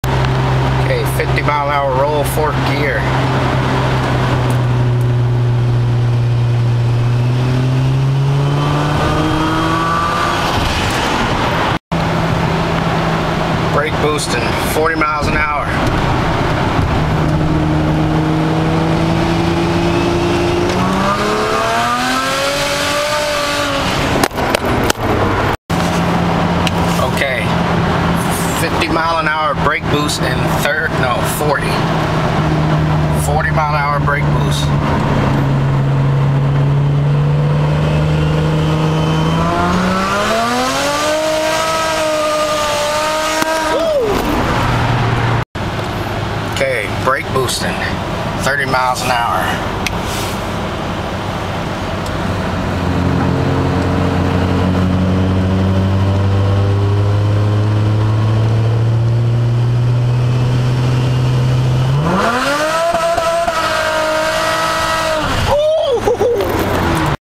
2002 Turbo Maxima DBW Brake Boost
For those that dont know on DBW cars limit the throttle opening when ECU senses your foot on the brake. So I wired a switch in so that when I want I can brake boost. Here are the results!!